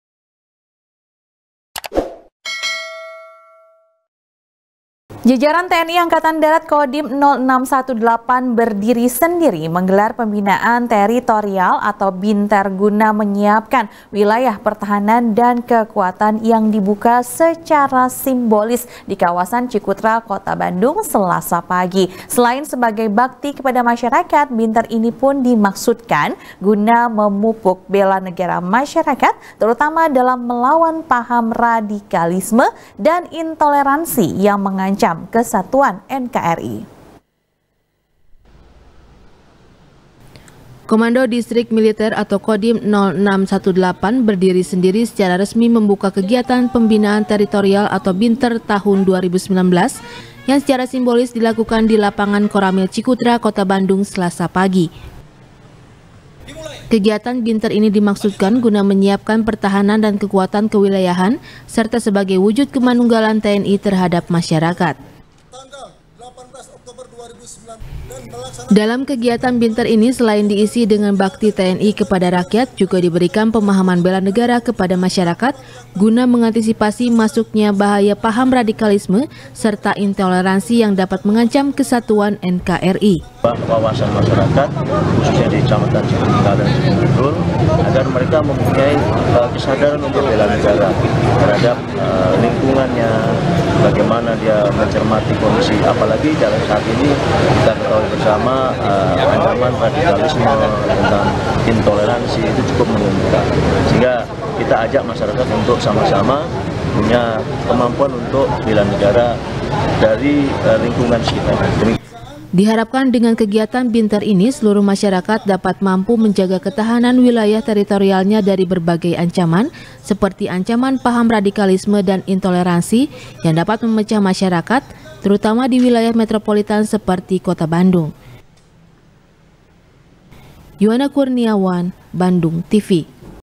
TNI Gelar Binter Cegah Paham Radikalisme Dan Intoleransi | SBR | BANDUNGTV
SBR | BANDUNGTV | KRISANTY | Jajaran TNI Angkatan Darat Kodim 0618 Berdiri Sendiri,Menggelar Pembinaan Teritorial Atau Binter Guna Menyiapkan Wilayah Pertahanan Dan Kekuatan,Yang Di Buka Secara Simbolis Di Kawasan Cikutra Kota Bandung,Selasa Pagi.Selain Sebagai Bakti Kepada Masyarakat,Binter Ini Pun Dimaksudkan Guna Memupuk Bela Negara Masyarakat,Terutama Dalam Melawan Paham Radikalisme Dan Intoleransi Yang Mengancam Kesatuan NKRI.